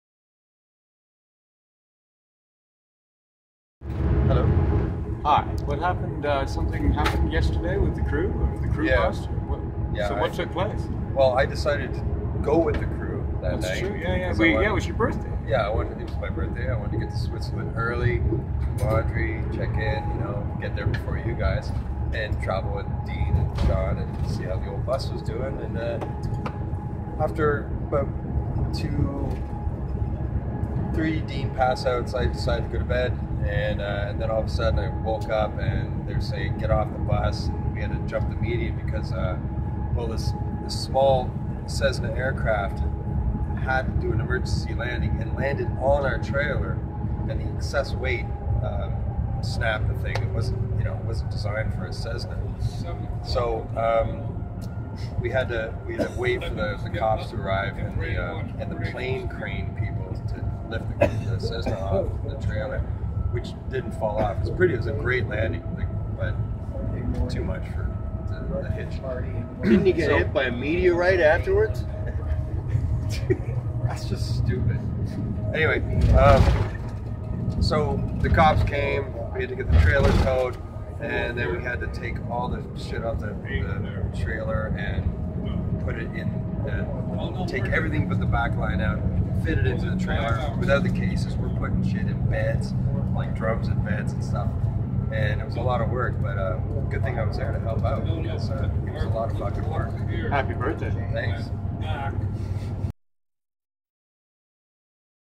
SAGA FCT Road Stories Mike Thorne Version
Mike Thorne shares his account of our autobahn mishap on the night of October 27th.